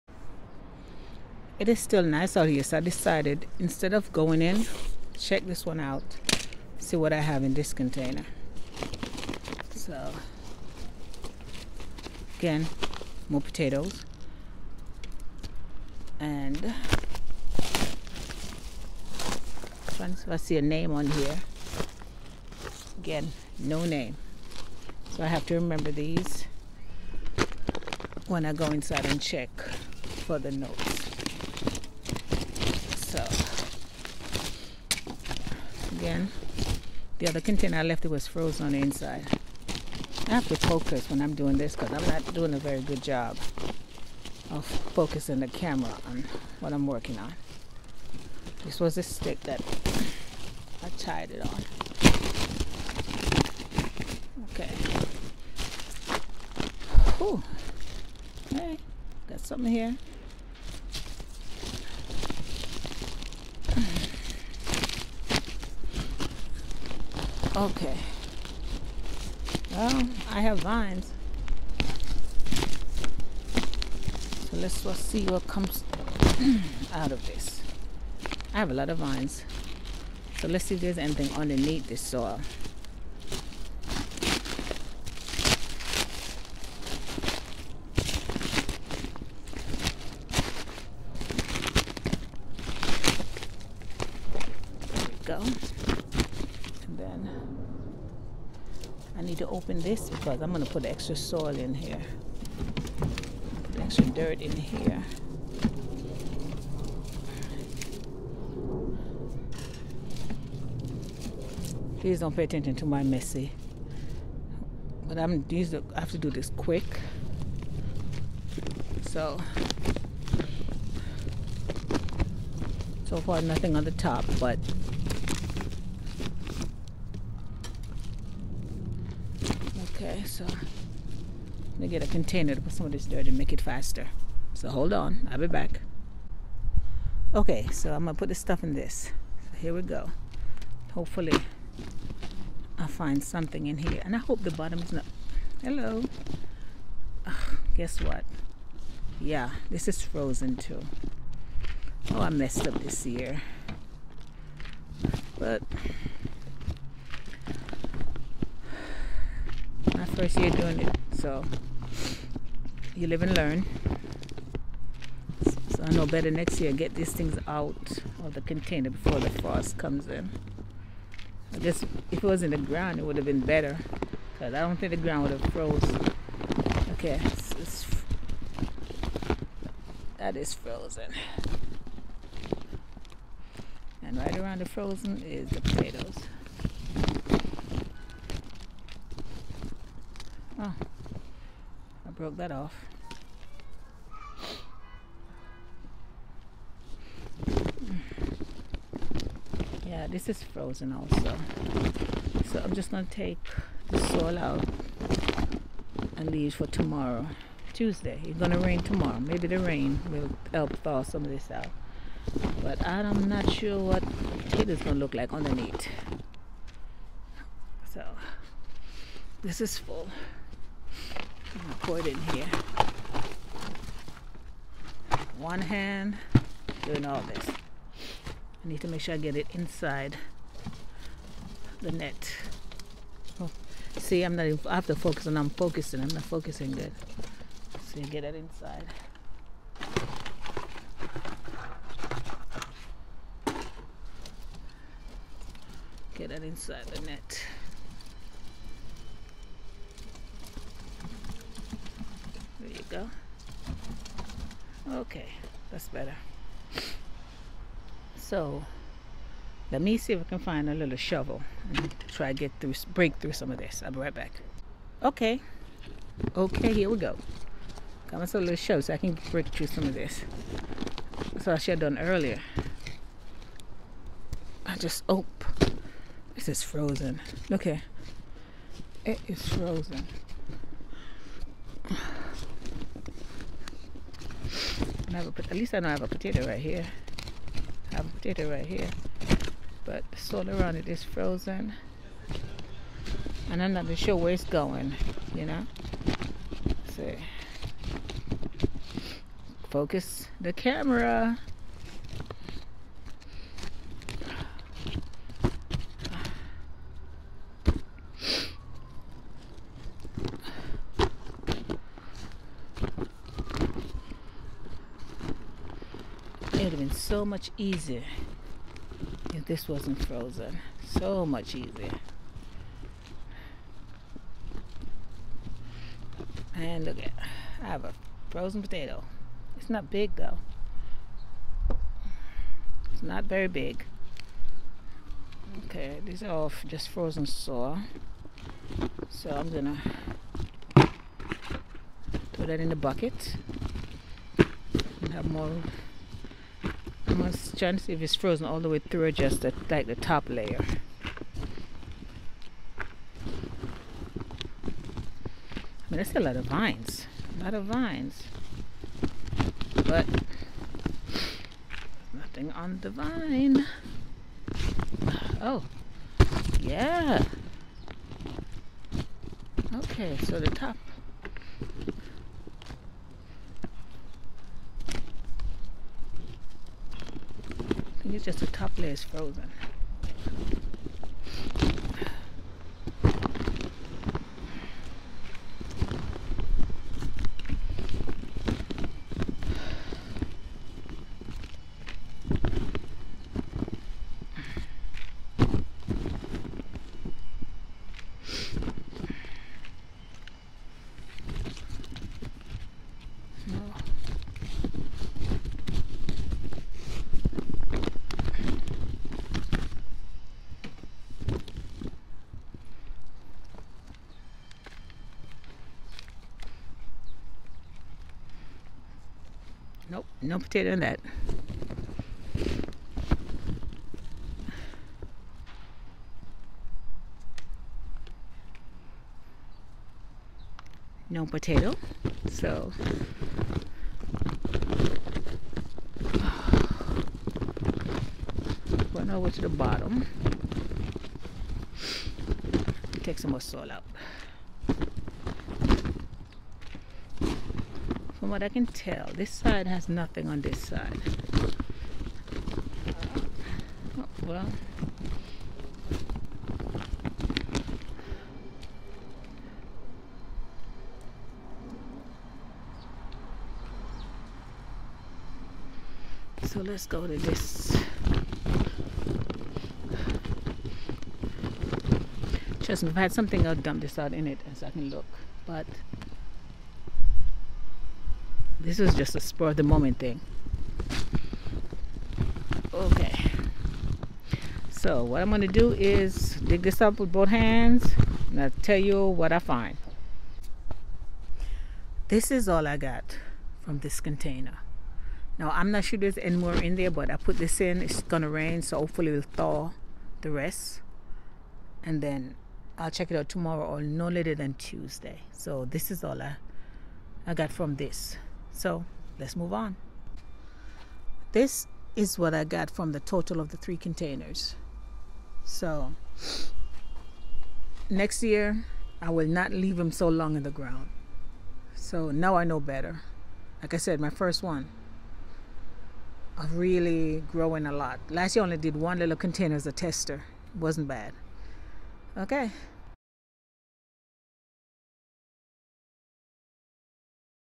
I Messed Up: Part 2. It's All About The Potatoes From My Container Vegetable Garden Part 3. Y1 V34.
This is video #3 of 5 from my potatoes series, but this is also video #2 of how I messed up my potatoes, from my 2018, container vegetable garden. I allowed them to get frozen instead of harvesting them before the first frost. I forgot these were tropical vegetables, and do not like frost. My fingers were numbed from digging in the cold, semi frozen soil. homegrown. potatoesincontainers. vegetablesincontainers. growingincontainers. letsstartsowing. smallgarden.